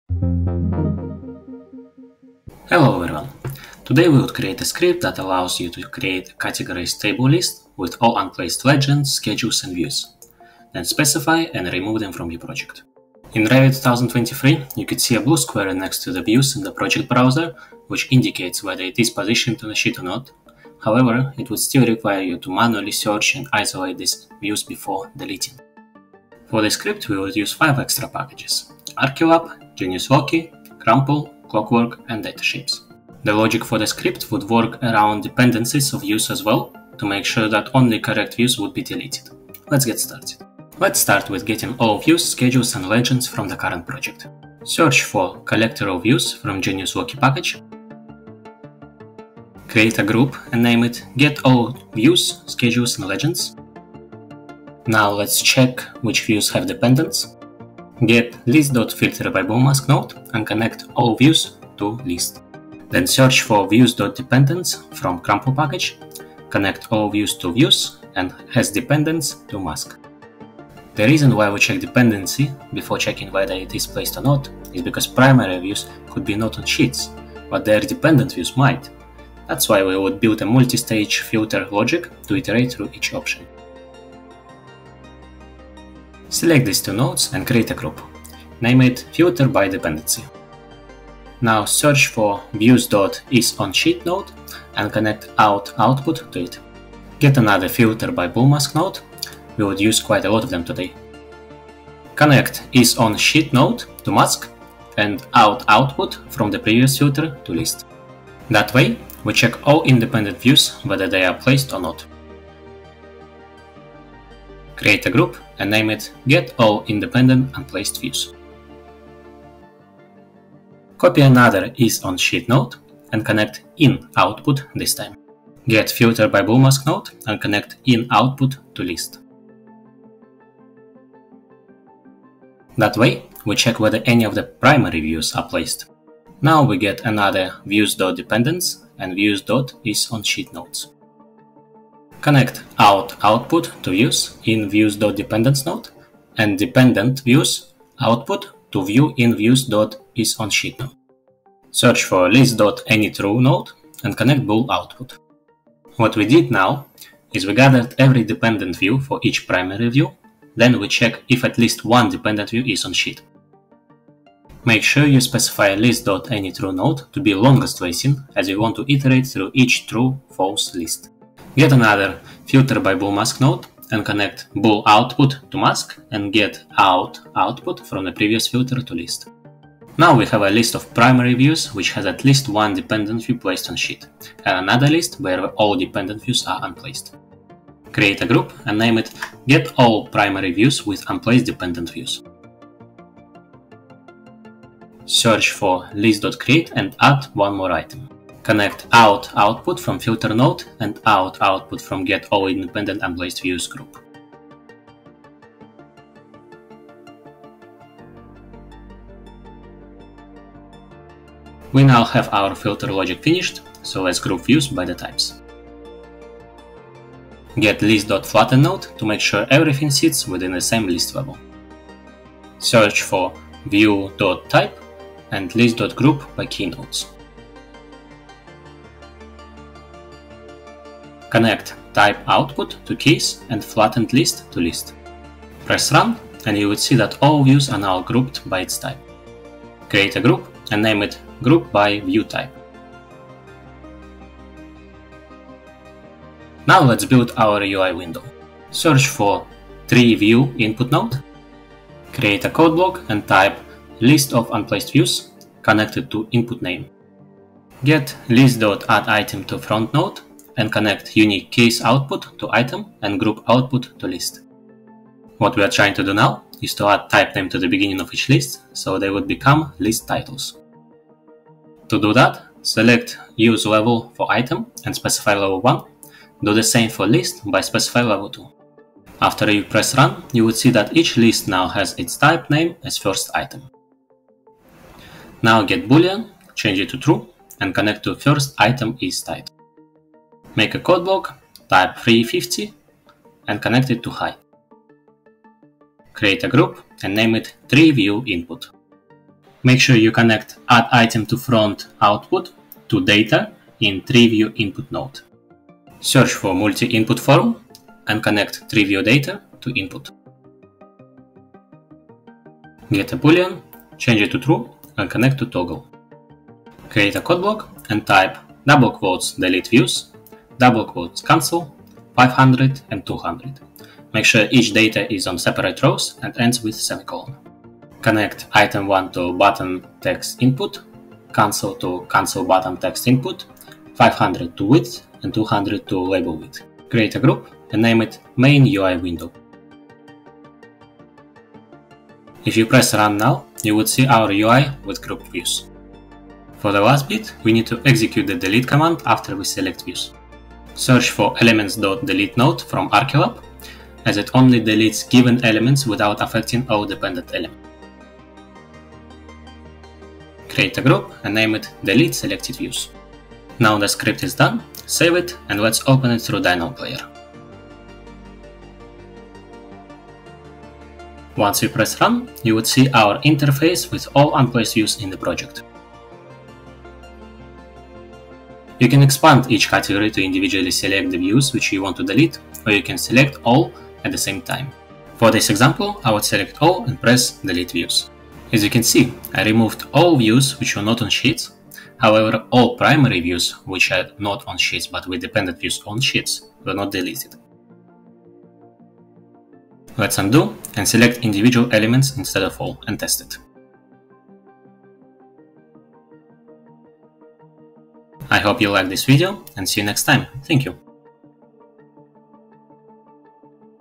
Dynamo - Delete Unused Views, Schedules and Legends. [Revit 2023]
Across different stages of the project, the Revit model can become full of test and unused views. At one moment it would become very confusing to navigate around the project browser.
It is especially challenging when you have many Primary and Dependent Views.

That is why today our script would:

° Generate a List of Unused Views, sorted by View Types.
° Select all or specific Views, Legends, and Schedules.
° Remove them from the Project, without affecting Dependency structure.

0:00 Intro.
0:27 Extra Packages.
0:40 Start Building the Script
6:10 Test with Dynamo Player.